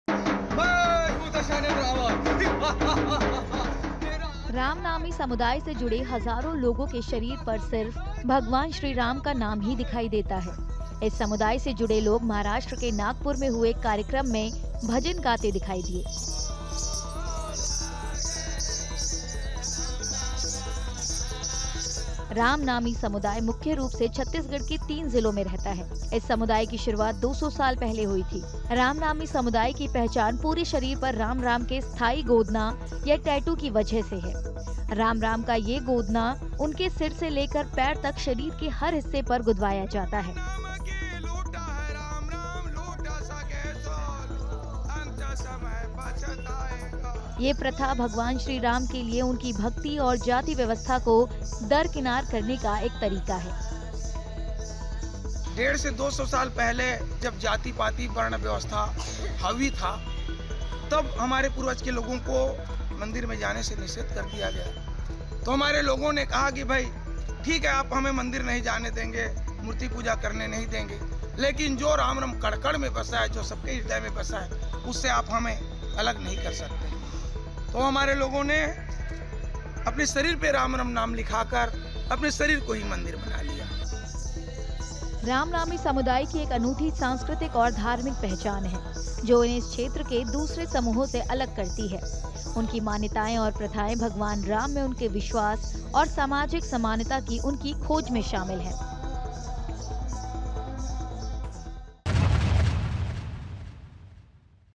Ram Mandir: भगवान राम की भक्ति में डूबा ये समुदाय, पूरे शरीर पर गुदवा रखे हैं Ram-Ram के टैटू
रामनामी समुदाय से जुड़े हजारों लोगों के शरीर पर सिर्फ भगवान श्रीराम का नाम ही दिखाई देता है। इस समुदाय से जुड़े लोग महाराष्ट्र के नागपुर में हुए एक कार्यक्रम में भजन गाते दिखाई दिए।रामनामी समुदाय मुख्य रूप से छत्तीसगढ़ के तीन जिलों में रहता है। इस समुदाय की शुरूआत 200 साल पहले हुई थी। रामनामी समुदाय की पहचान पूरे शरीर पर राम-राम के स्थाई गोदना या टैटू की वजह से है। राम-राम का ये गोदना उनके सिर से लेकर पैर तक शरीर के हर हिस्से में गुदवाया जाता है..